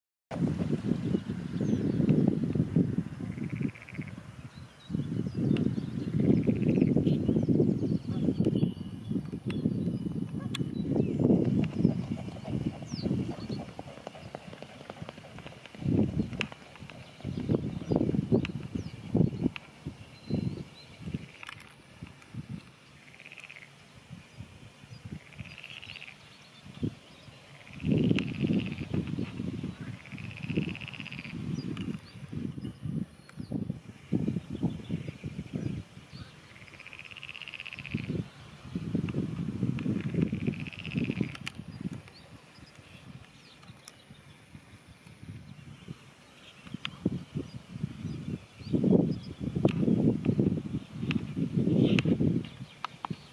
Vader zwaan jaagt Canadese gans uit het water.
Een mannetjeszwaan bewaakt zijn gezin (vrouwtje met 5 jongen) in een plas en duld geen andere watervogels in diezelfde plas. Deze Canadese gans wordt zonder pardon weggebonjourd!